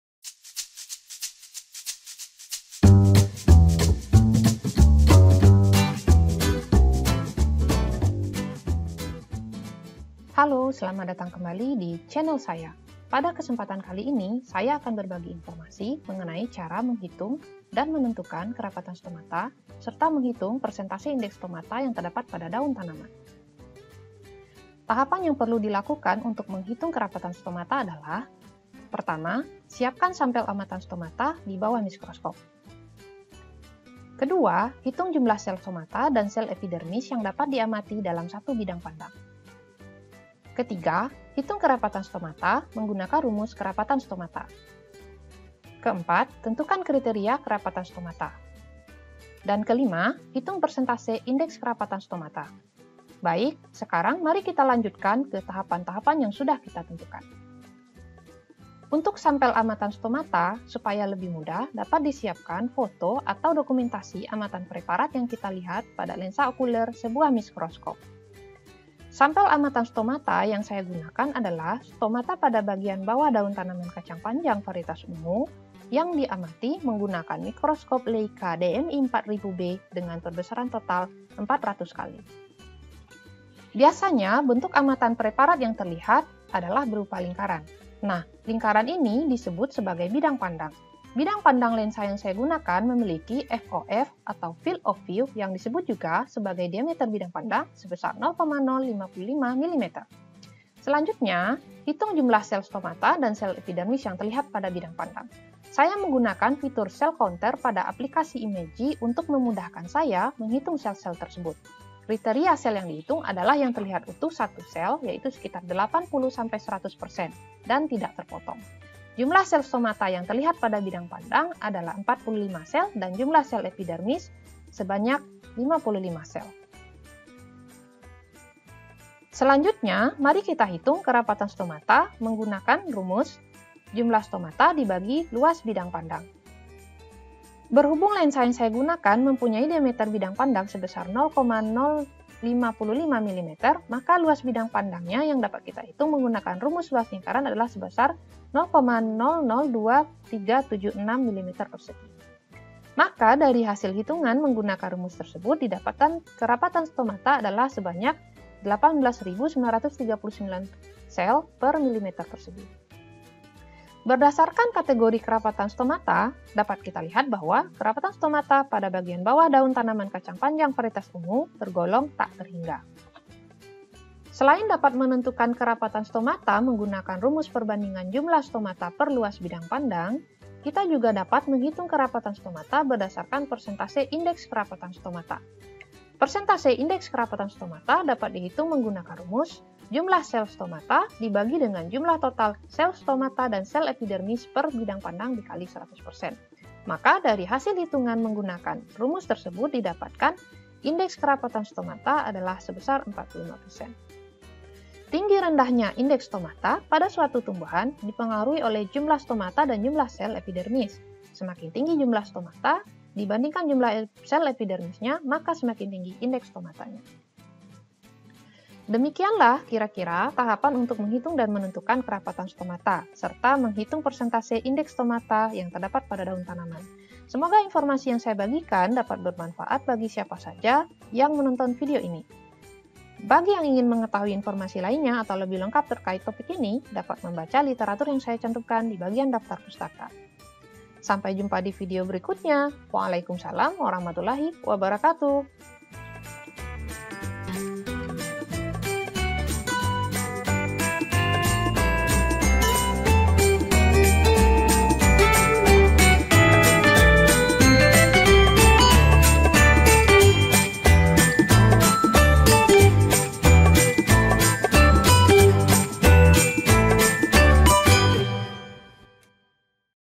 [Tutorial] Menghitung Kerapatan, Menentukan Kriteria, dan Persentase Indeks Kerapatan Stomata
Video ini memuat informasi tentang cara menghitung kerapatan (sel/mm2), menentukan kriteria kerapatan dan persentase indeks kerapatan stomata (%).